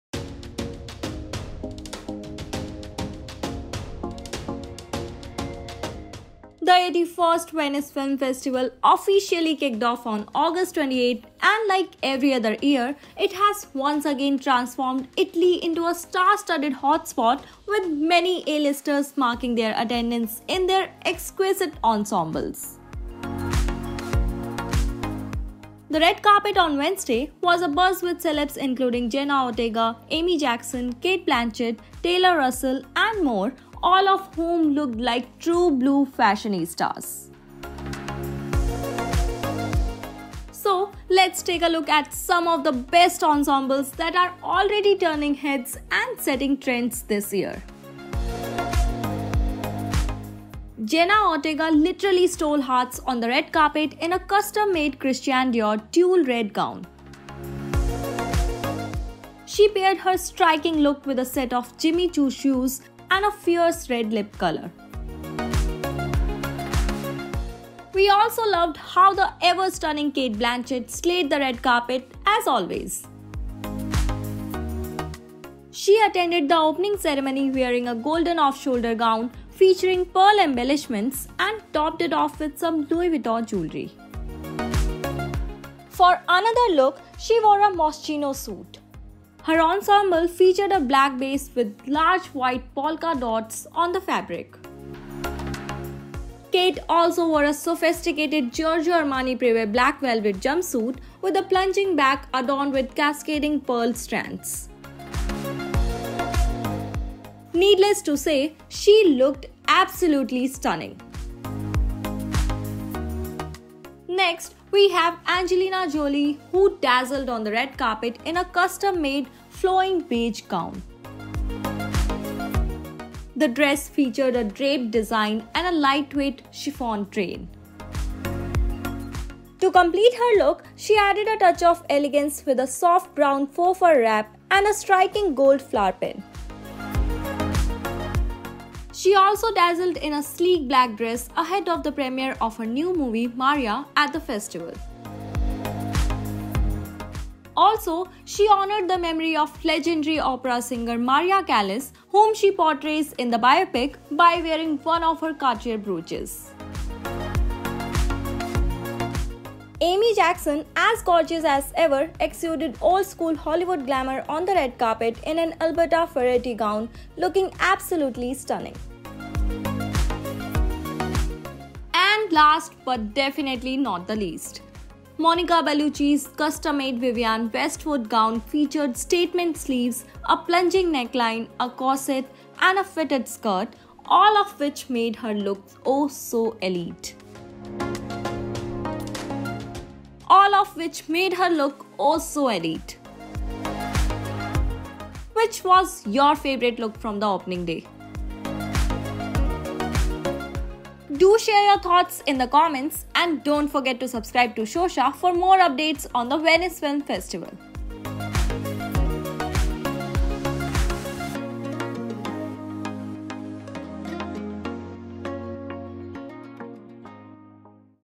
Venice Film Festival 2024: Angelina Jolie, Jenna Ortega & MORE Turn Heads With Their Fashion | N18G
The 81st Venice Film Festival, where Hollywood's elite gather to celebrate cinematic excellence and showcase their impeccable style, has already begun. From Jenna Ortega's daring ensemble to Amy Jackson's elegant gown and Cate Blanchett's timeless sophistication, let's take a look at some of the best fashion moments that are already setting trends and turning heads this year. Watch this video to know more.